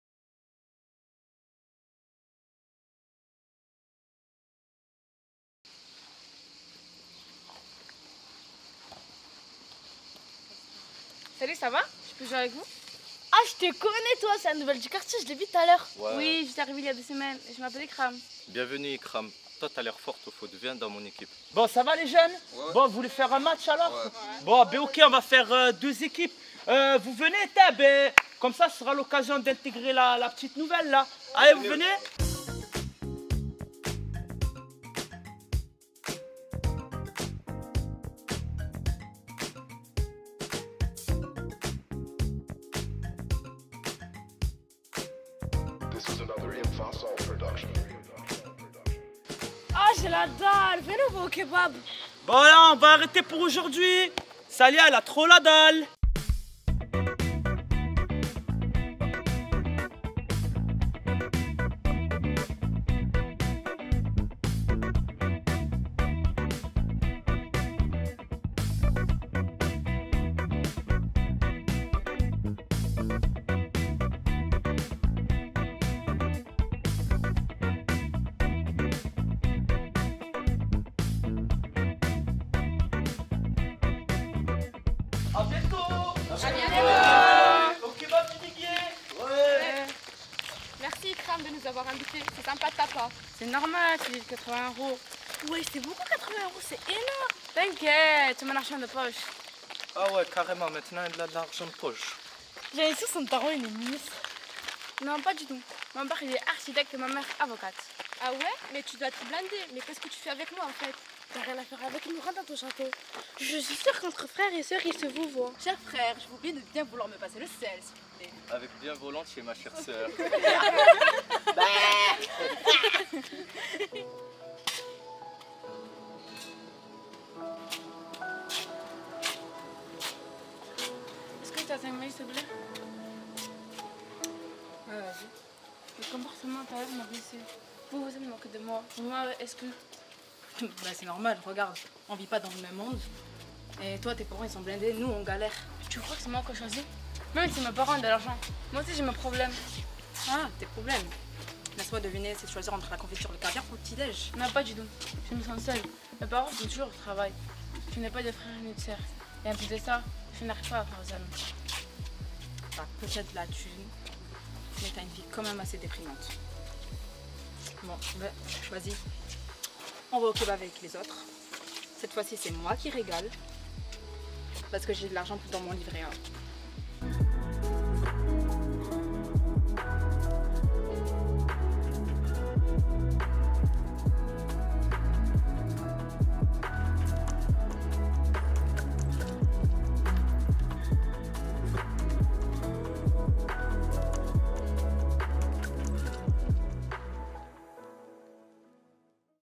histoire d'exclusion
En ce jour de rentrée scolaire , certains écoliers, collégiens et lycéens vont se rendre peut-être dans un nouvel établissement qu'ils ne connaissent pas où déja des groupes de jeunes se connaissent depuis plusieurs années.  Parfois pour un nouveau, il est très difficile de se faire intègrer,apprécier et respecter tout en gardant ses propres opinons et sans cacher le milieu d'où il vient.
Afin de prendre conscience que le vivre et faire ensemble est important les élèves de l'école karaté karaté omnisports viguier organisé sous la direction de 6céron vous présente cette histoire d'exclusion. N'oublions pas notre devise :Savoir vivre et faire ensemble pour réussir ensemble !